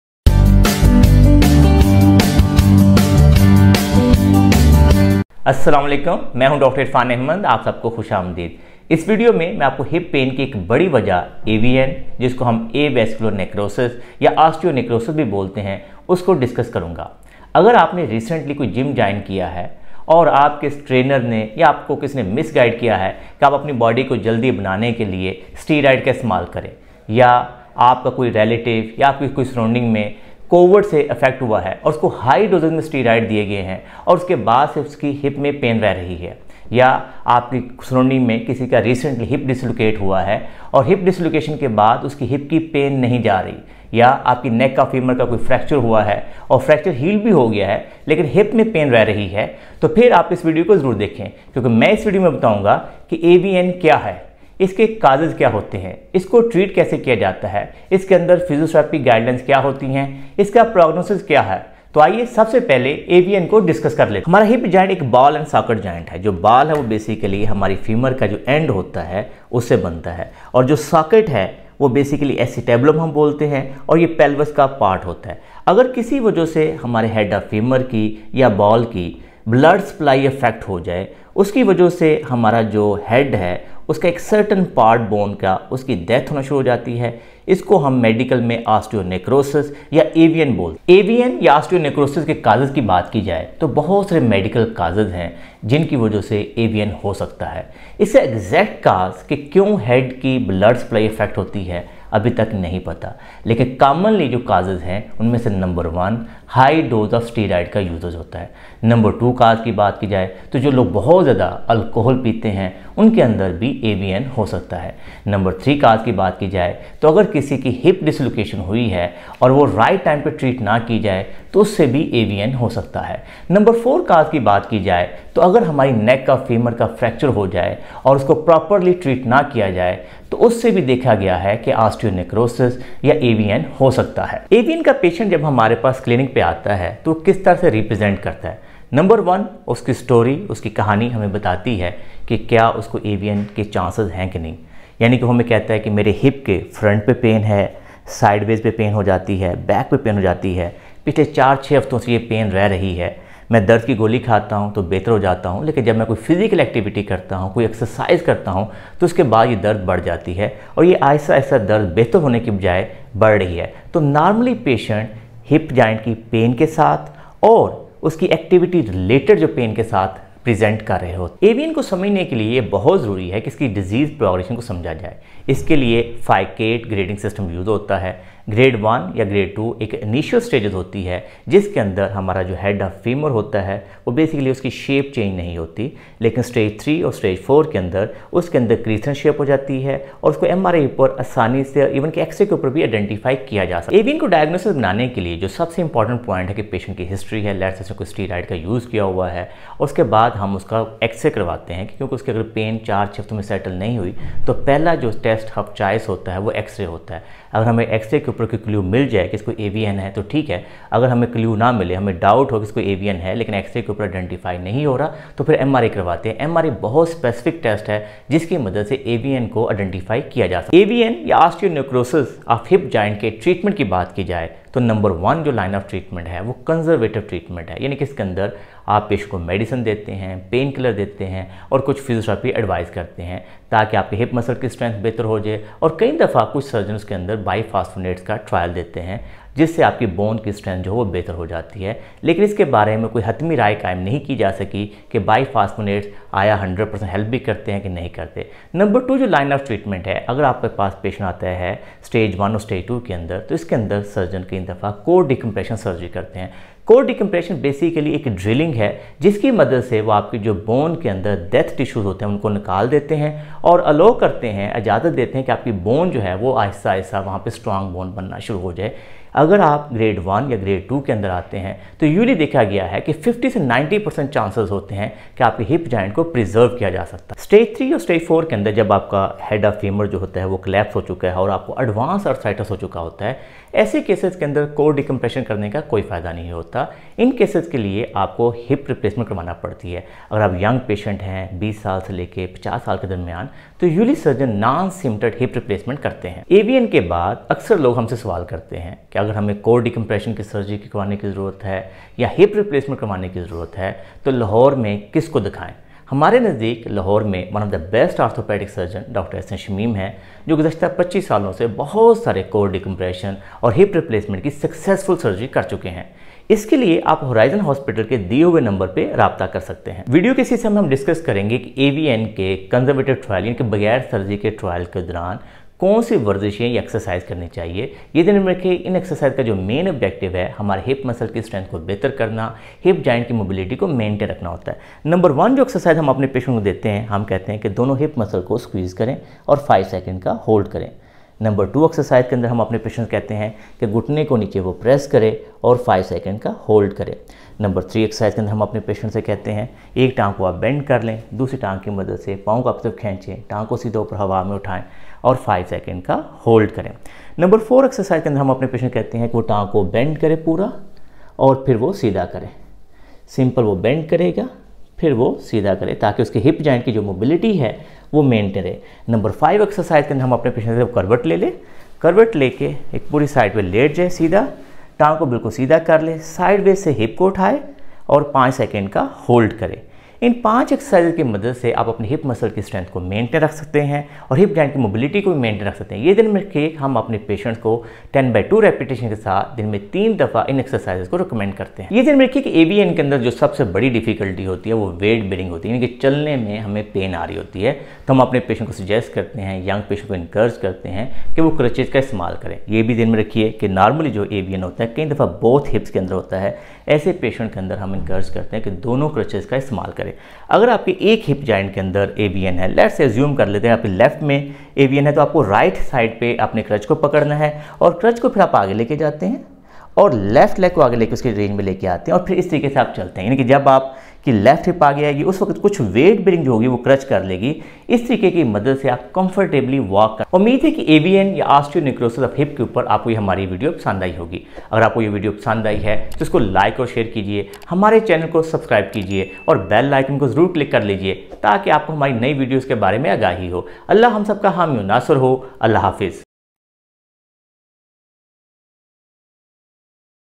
Learn About Hip Pain Due to AVN| Causes ,Treatment and Exercises For AVN of Hip Joint|Urdu|Hindi
In this Video Dr Irfan Ahmed Consultant Orthopaedic and Sports Physiotherapist  will discuss  Hip Pain Due to AVN or Avascular necrosis  or osteonecrosis of Head of femur .
In AVN blood supply  of head of femur is compromised which may leads towards death of  head or ball of femur .
common Causes of Osteonecrosis are
1.Overdose steroids
2.Over use of alcohol
3.hip Dislocation
4.Neck of femur  fracture
Disease  progression is monitored  througFicat 4 stage  method.
Treatment
1.conservative
which may includes Pain killers ,Biphosphonates, active rest and physiotherapy
2.core decompression
3. Hip replacement